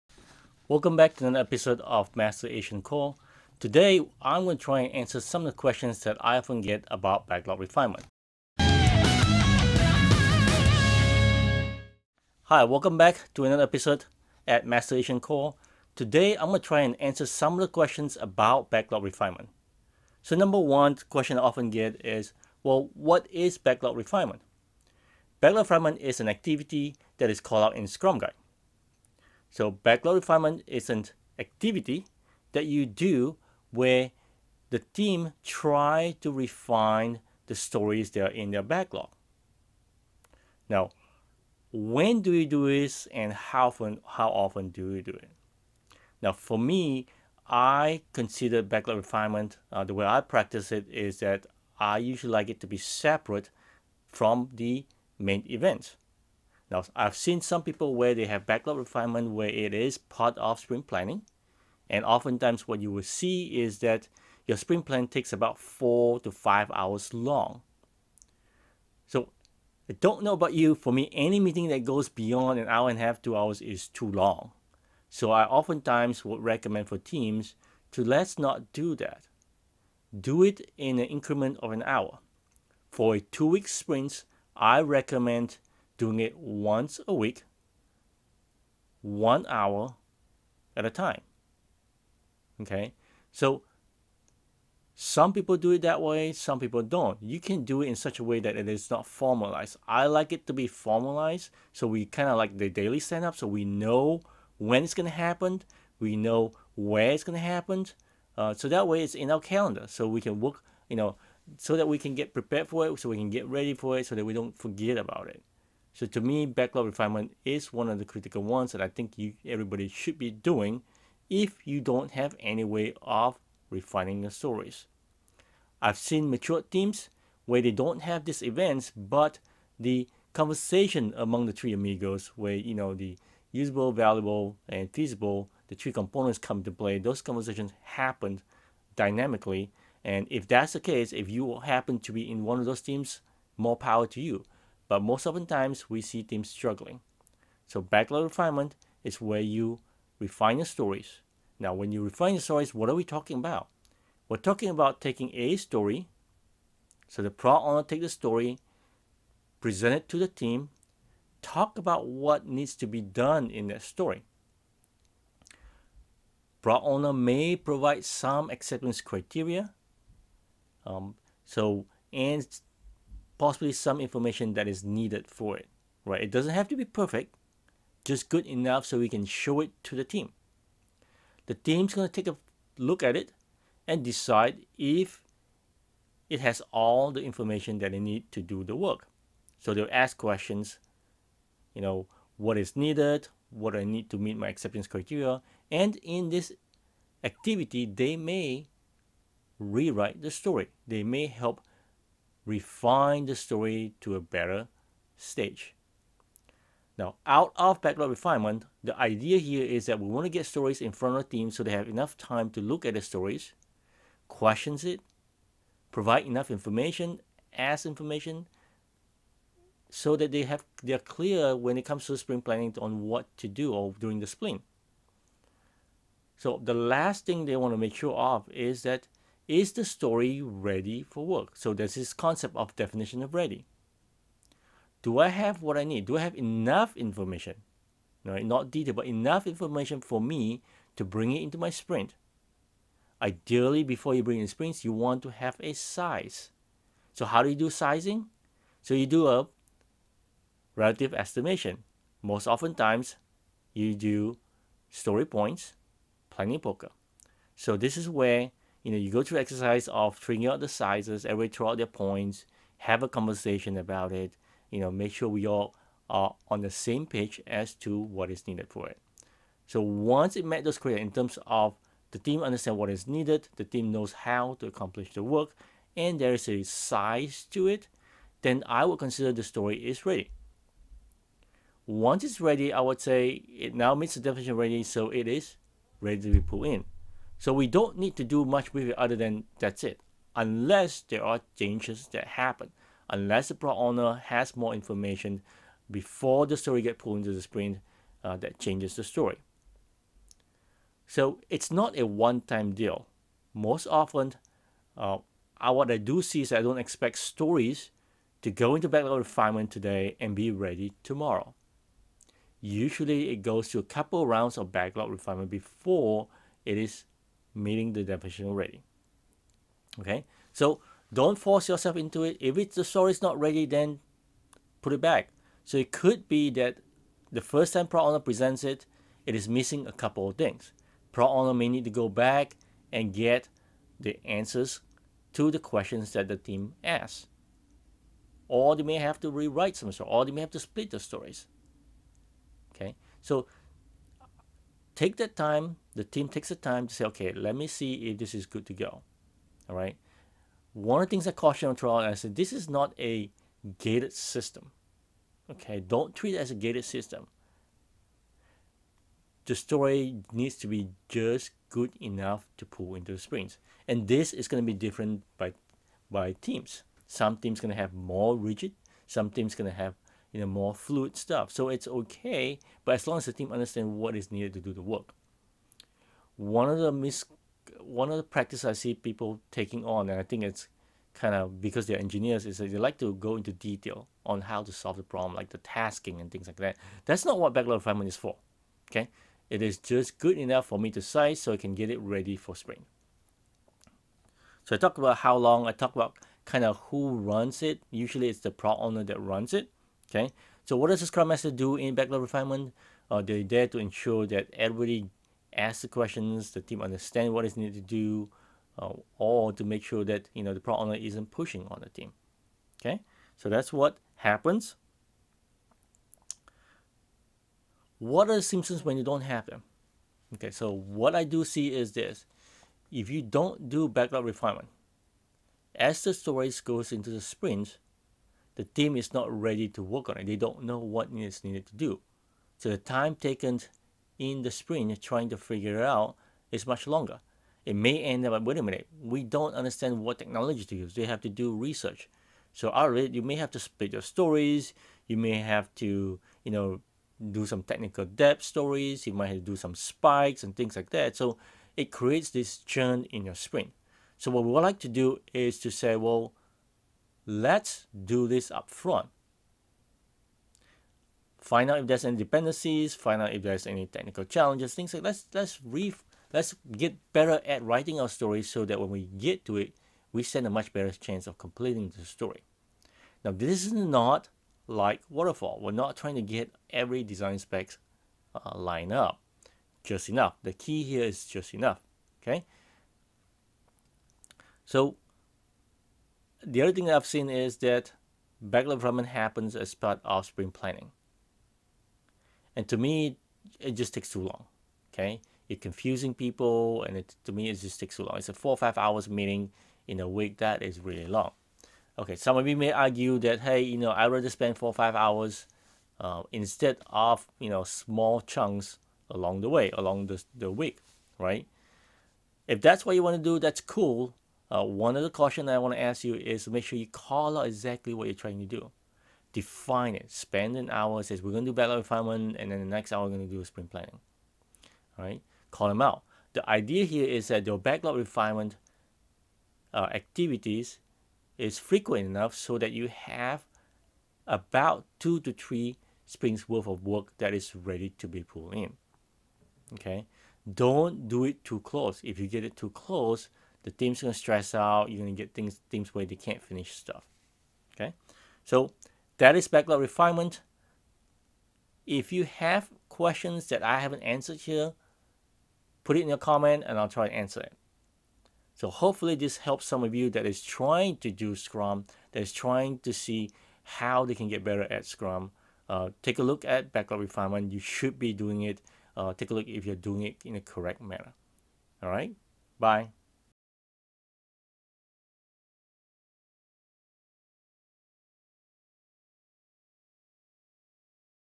Questions about Backlog Refinement.
Are you doing Backlog Refinement? Don't know what it is? In this episode I answer some common question that I get about Backlog Refinement.